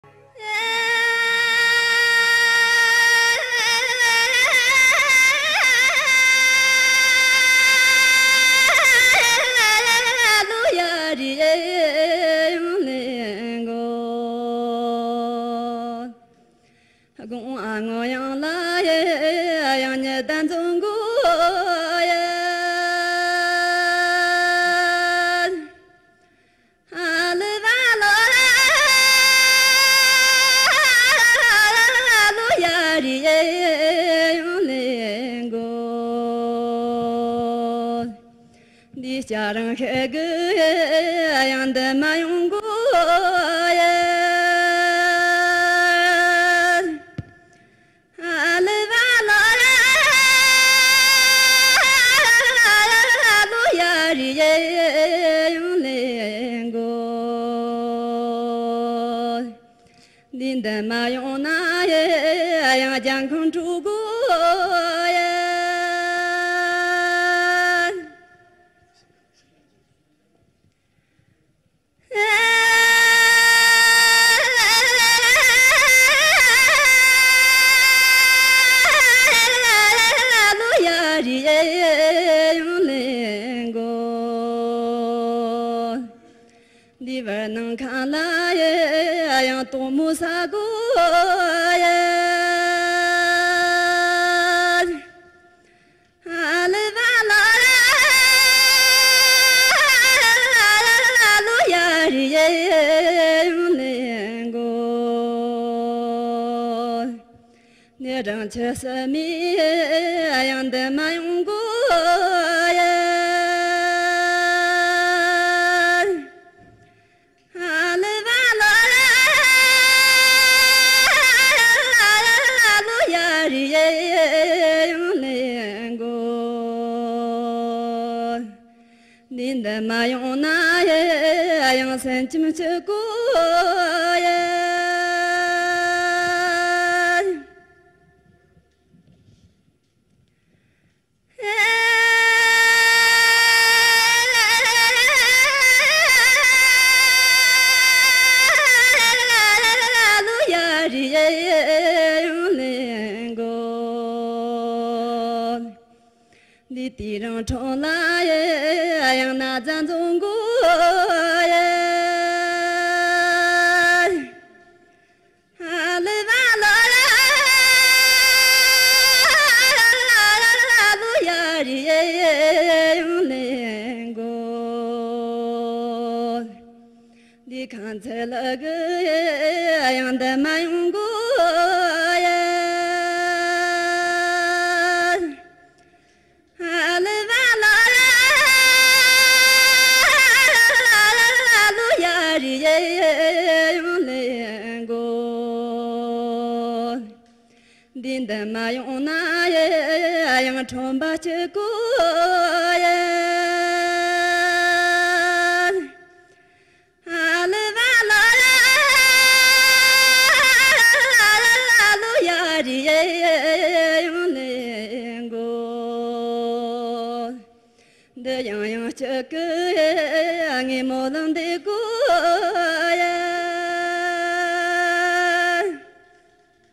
The First Tibetan Traditional Song Contest (2)
This traditional song contest was held in August 16, 2007 in Xining (Zi ling) City, Qinghai (Mtsho sngon) Province, by the Qinghai Provincial Cultural Bureau, Qinghai TV Station, Qinghai People's Broadcast Station and Himalaya Cultural Commerce Limited Corporation. It was published by Beijing Zhongti Audio-video Publishing Center. This video also contains singers' names and song titles.